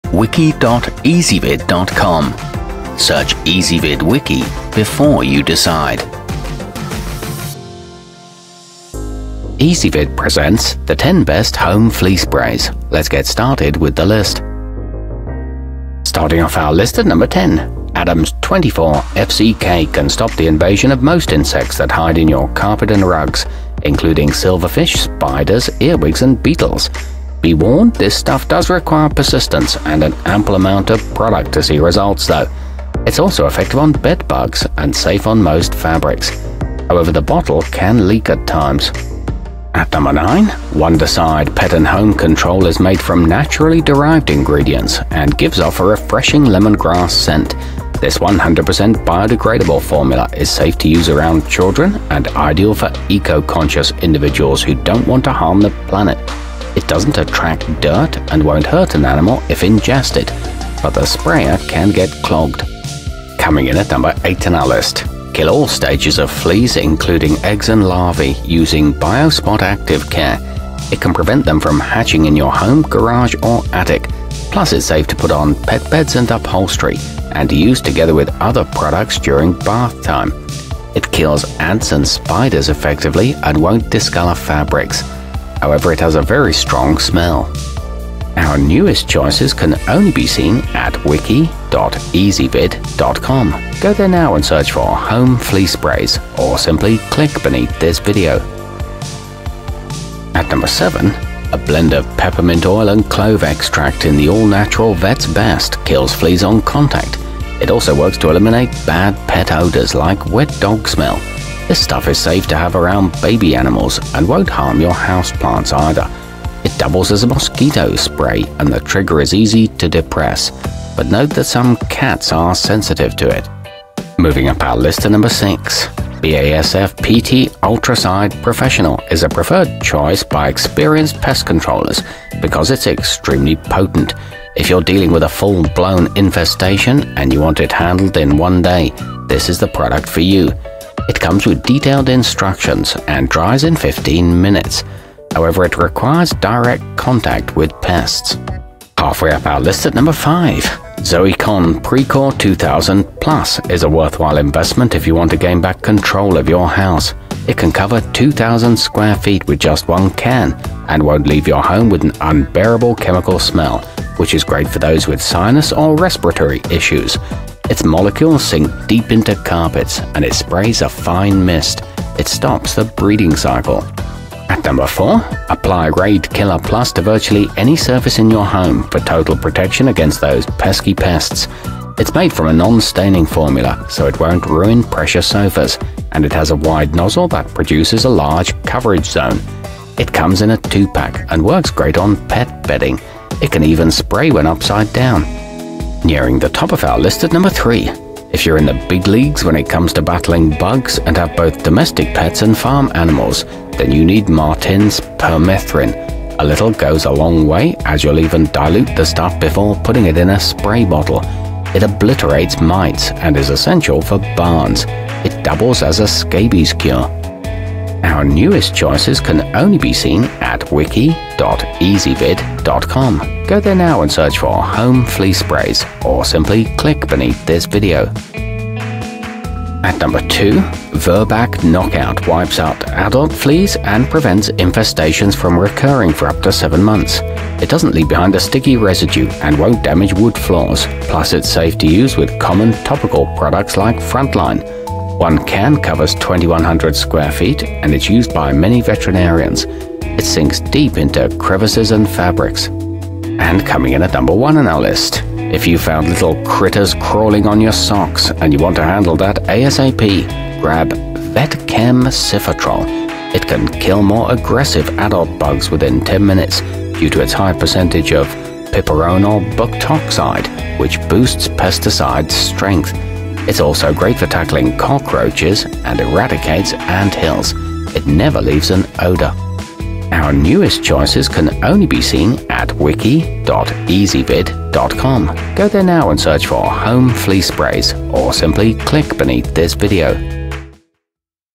10 Best Home Flea Sprays 2018
Our complete review, including our selection for the year's best home flea spray, is exclusively available on Ezvid Wiki.

Home flea sprays included in this wiki include the vet's best, adams 24 fzk, vet kem siphotrol, basf pt ultracide professional, wondercide pet & home control, virbac knockout, zoecon precor 2000 plus, biospot active care, raid killer plus, and martin's permethrin.